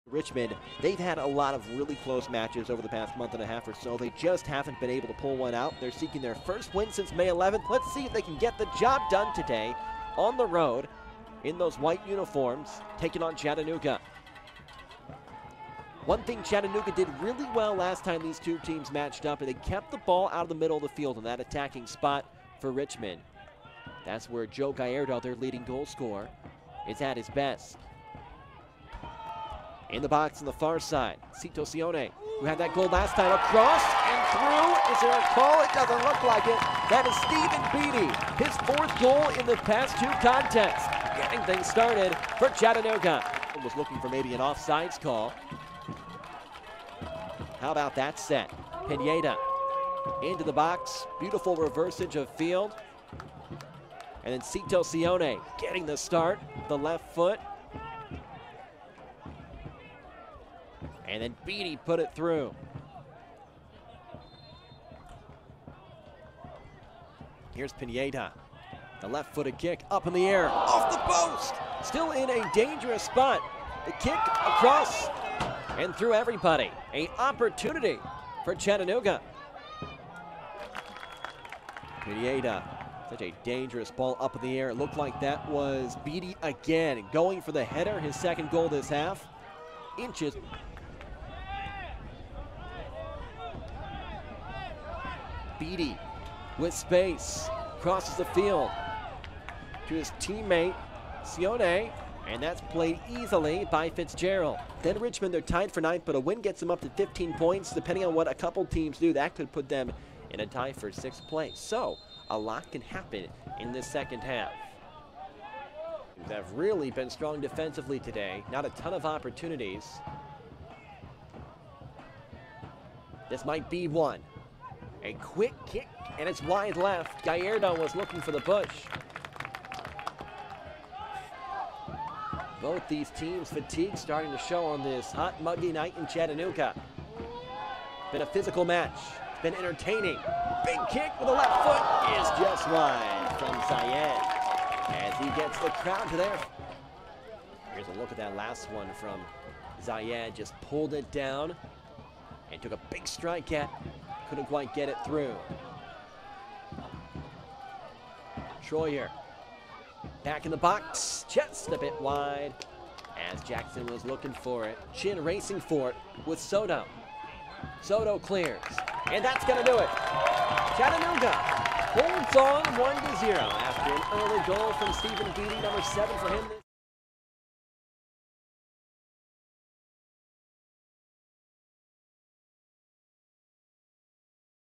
HIGHLIGHTS Chattanooga Red Wolves SC vs Richmond Kickers
Chattanooga scored in the seventh minute as the Kickers fell on the road